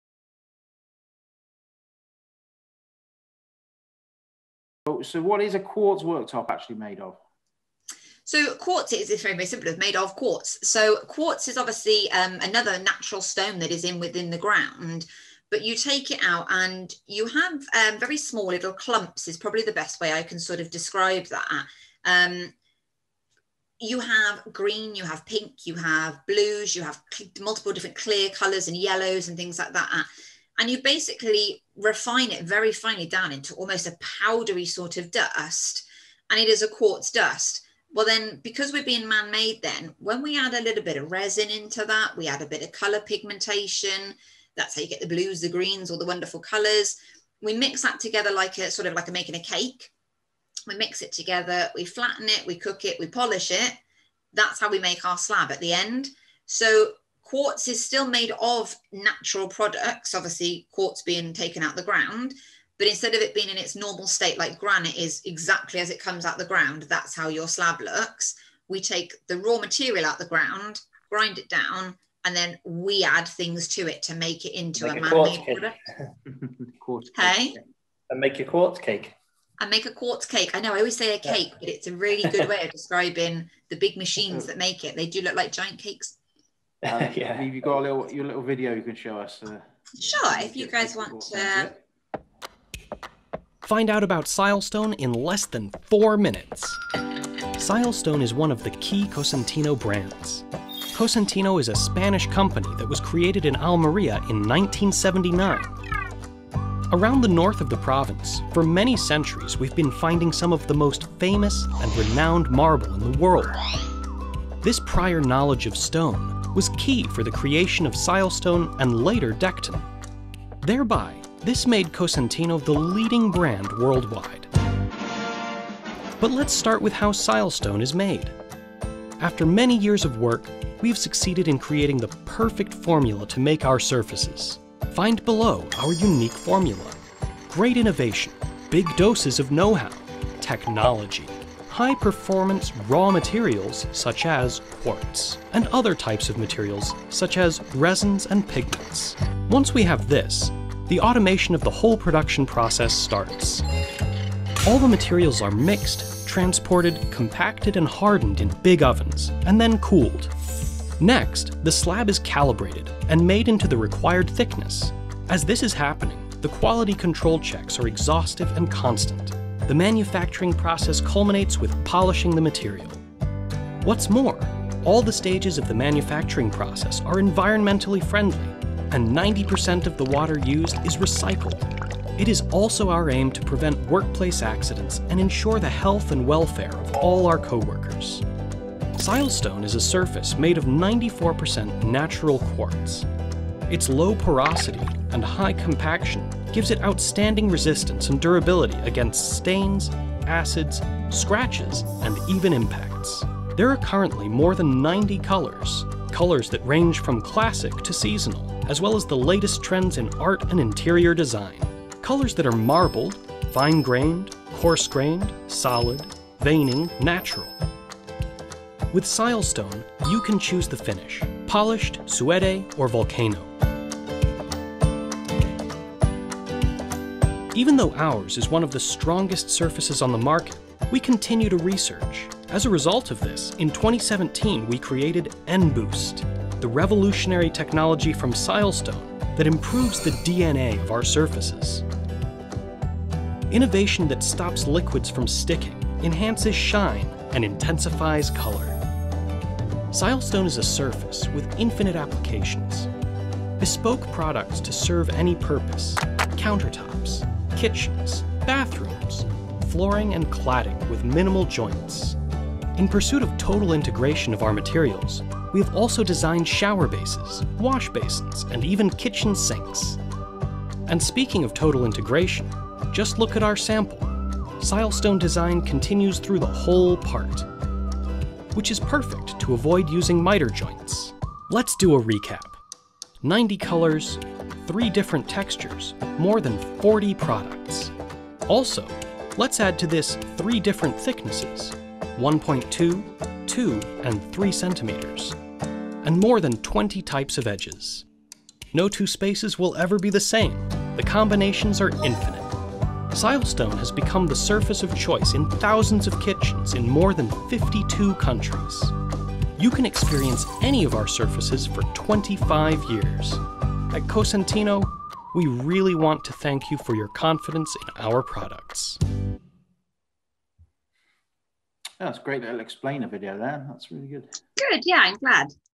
What Are Quartz Worktops Made Of?
During our Kitchen Design Podcast we asked Katie Stone from Cosentino, the world's leading stone worktop manufacturer, "What Are Quartz Worktops Made Of?"... Here's what she had to say.